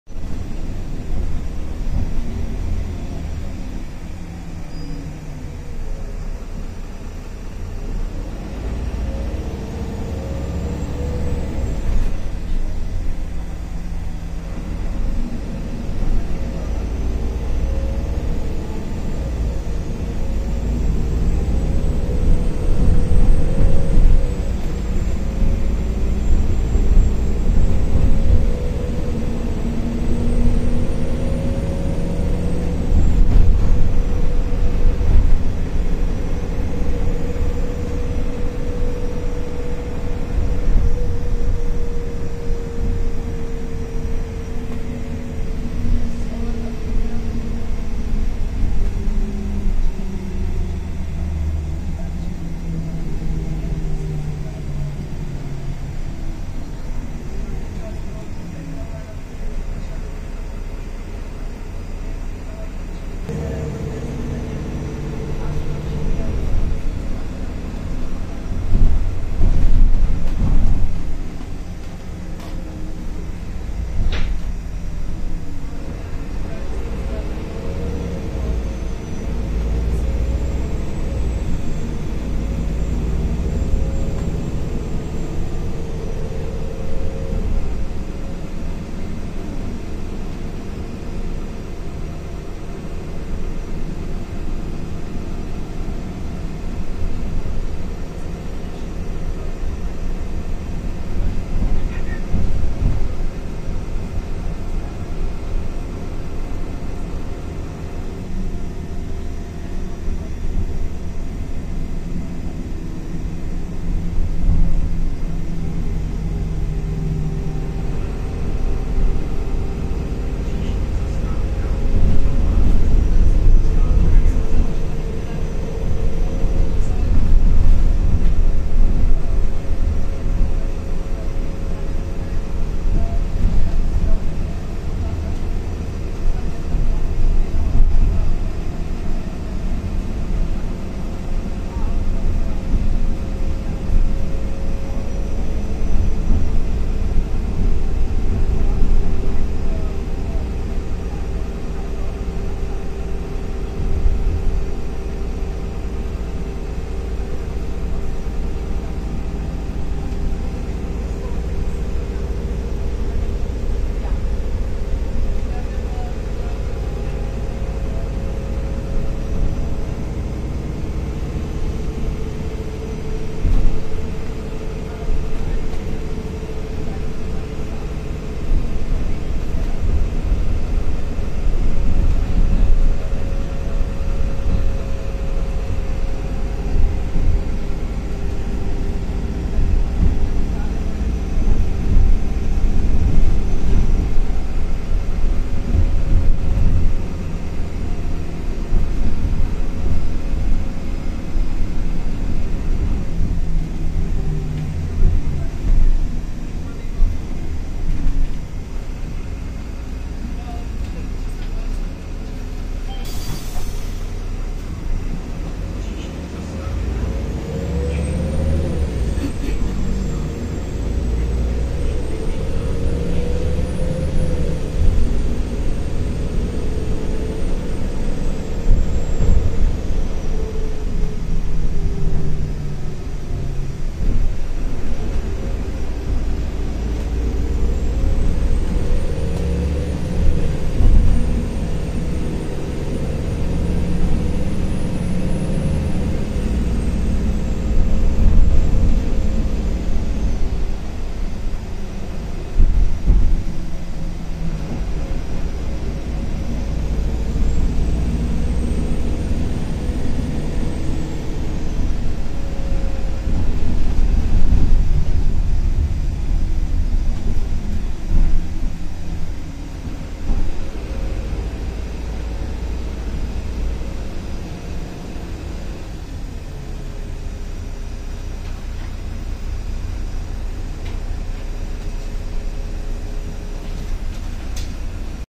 Jízda autobusem Irisbus Crossway LE 12M ev.č. 7812 - DPMB (část I.)
Jízda brněnským autobusem Irisbus Crossway LE 12M, evidenční číslo 7812, rok výroby 2010, na lince 302 (kurz 06508, Zoo - Kníničky), dne 9.1.2024.